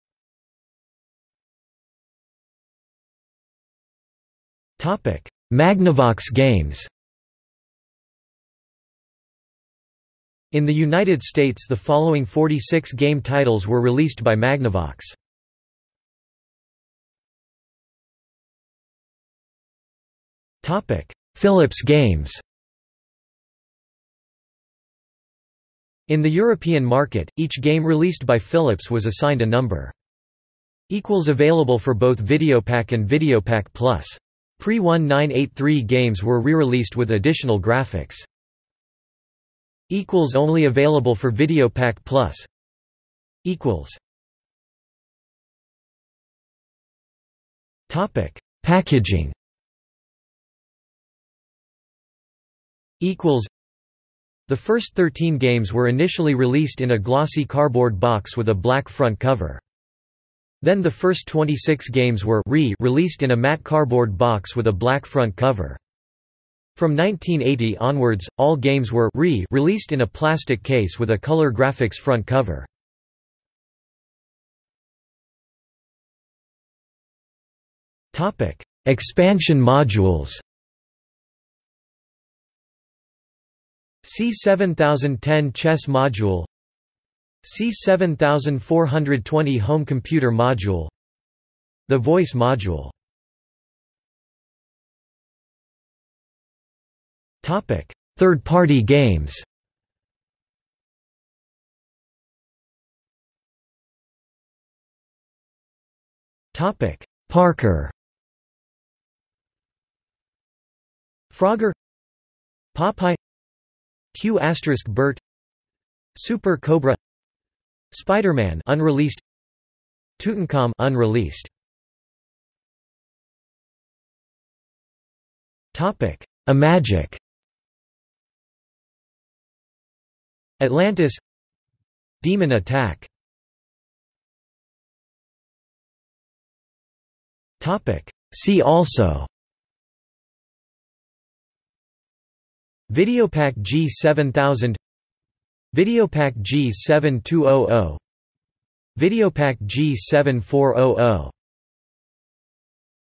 List of Magnavox Odyssey² games | Wikipedia audio article
00:00:01 1 Magnavox games
00:00:17 2 Philips games
00:00:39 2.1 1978sup&#91;4&#93;/sup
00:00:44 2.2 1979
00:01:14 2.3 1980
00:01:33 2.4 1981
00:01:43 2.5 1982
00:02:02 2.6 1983
00:02:14 2.7 Packagingsup&#91;6&#93;/sup

- Socrates